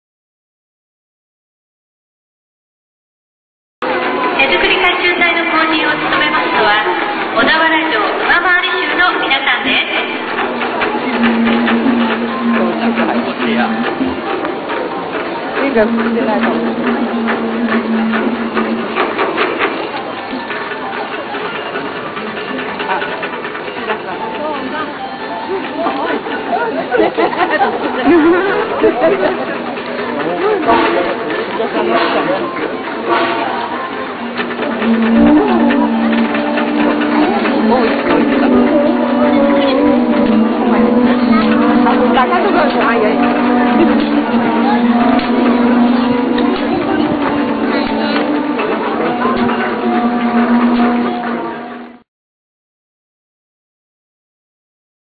北条手作り甲冑隊 ＠北條五代祭り 小田原
2009年5月3日小田原城下で開催された「北條五代祭り」での、武者行列。「小田原北条手作り甲冑隊」の皆さんです。Warrior procession,The samurai's parade.